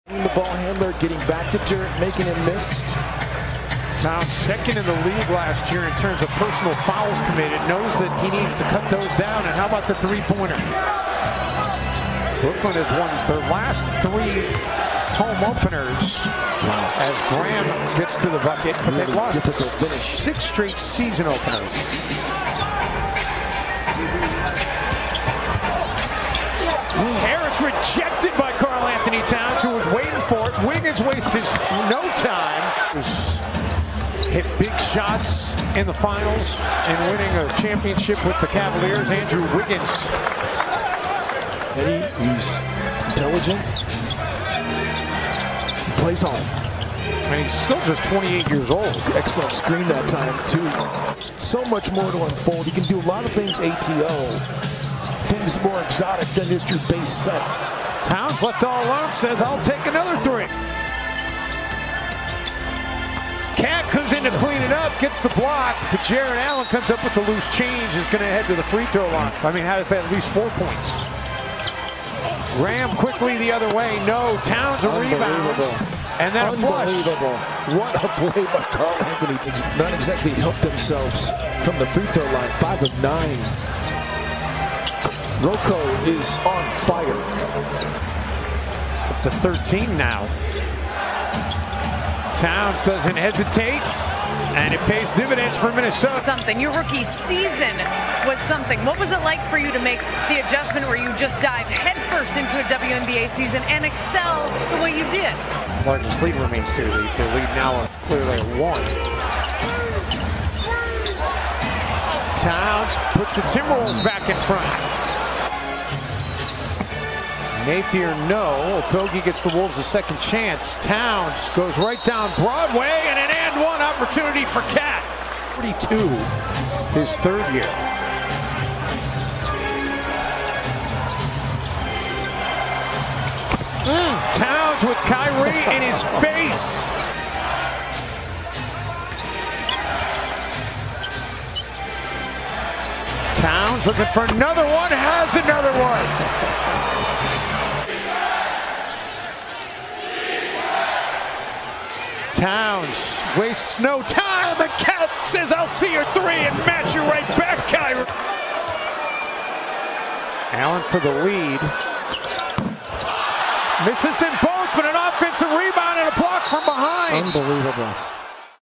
Highlights | Karl-Anthony Towns With 36 Points, 14 Rebounds, 3 Blocks, 3 Assists vs. Nets (10.23.19)
Timberwolves center Karl-Anthony Towns led Minnesota to a 127-126 overtime win over the Nets in the season opener to start the 2019-20 season.